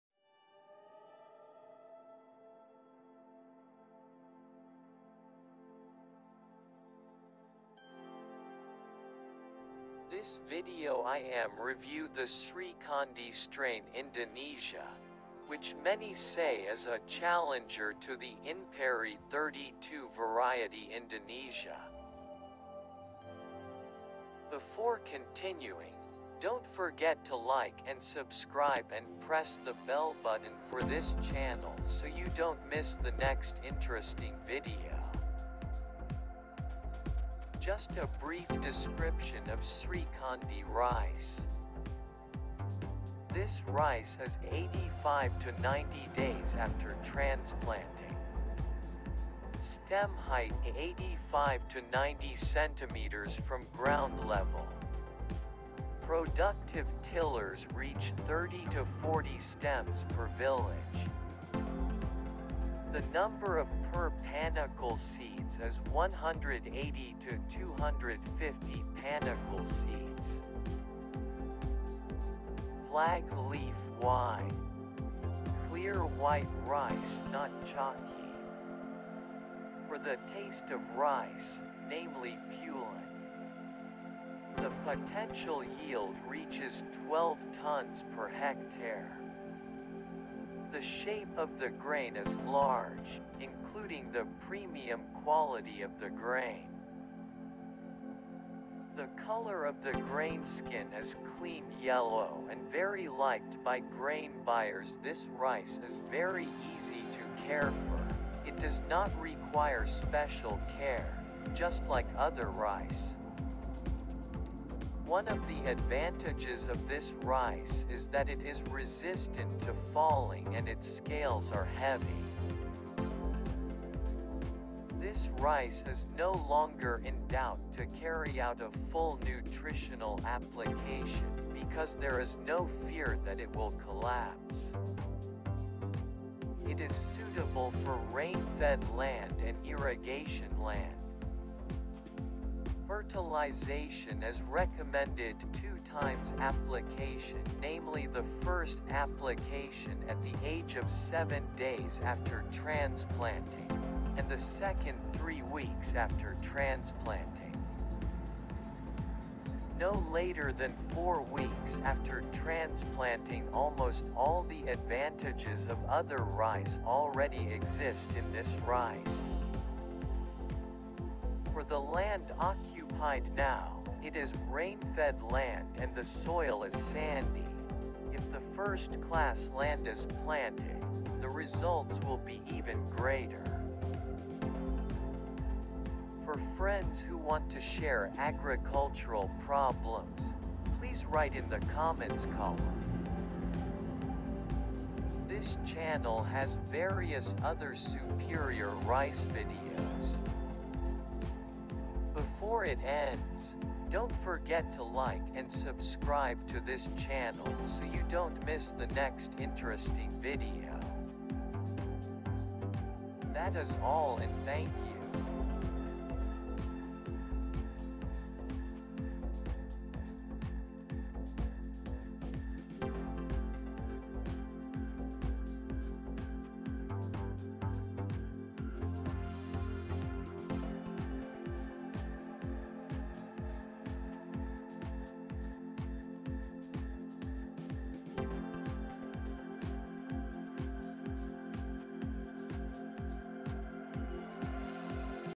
Indonesian Superior Rice || Srikandi rice
THIS VIDEO I AM REVIEWING THE SRIKANDI Strain, which many say is a challenger to the Inpari Tiga Dua variety.

just a brief description of SRIKANDI Rice.

This rice is 85 to 90 days after transplanting.

Stem height 85 to 90 cm from ground level.

Productive tillers reach 30 to 40 stems per village.

The number of permalay seeds is 180 to 250 permalay seeds.

Flag leaf wide.

clear white rice not chalky.

for the taste of rice, namely Pulen.

The potential yield reaches 12 tons per hectare.

the shape of the grain is large, including the premium quality of the grain.

The color of the grain skin is clean yellow and very liked by grain buyers

This rice is very easy to care for, it does not require special care, just like other rice.

One of the advantages of this rice is that it is resistant to falling and its scales are heavy.

This rice is no longer in doubt to carry out a full nutritional application because there is no fear that it will collapse.

It is suitable for rainfed land and irrigation land.

Fertilization is recommended 2 times application
namely the first application at the age of 7 days after transplanting, and the second 3 weeks after transplanting. no later than 4 weeks after transplanting

almost all the advantages of other rice already exist in this rice.

for the land occupied now, it is rainfed land and the soil is sandy, if the first-class land is planted, the results will be even greater.

For friends who want to share agricultural problems, please write in the comments column.
This channel has various other superior rice videos.

THAT IS ALL AND THANK YOU.
Petta lapute official